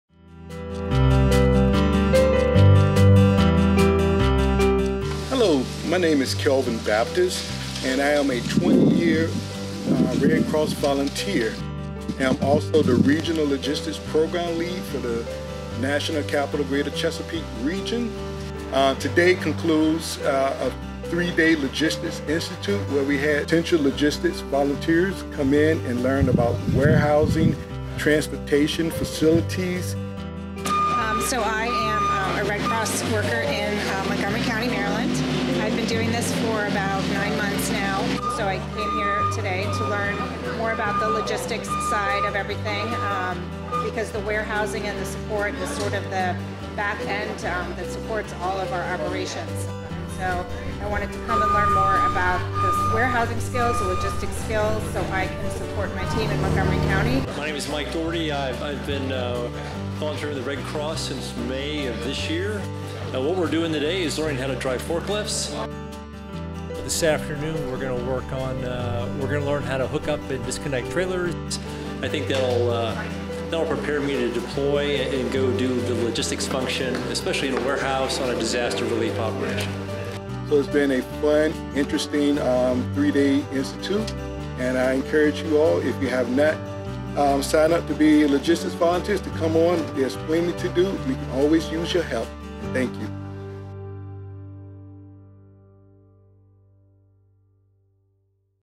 Transportation team training November 2023
The American Red Cross responds to a disaster every eight minutes and is ready 24/7.
An important part of this work is ensuring that we have vehicles and volunteers ready to roll! Thank you to the @RedCrossNCGC volunteers who spent a recent Saturday training in Gaithersburg, Maryland.
Interested in supporting your community (and learning skills like this)?
🎥 Video by RedCrossNCGC volunteer Cynthia Vaughn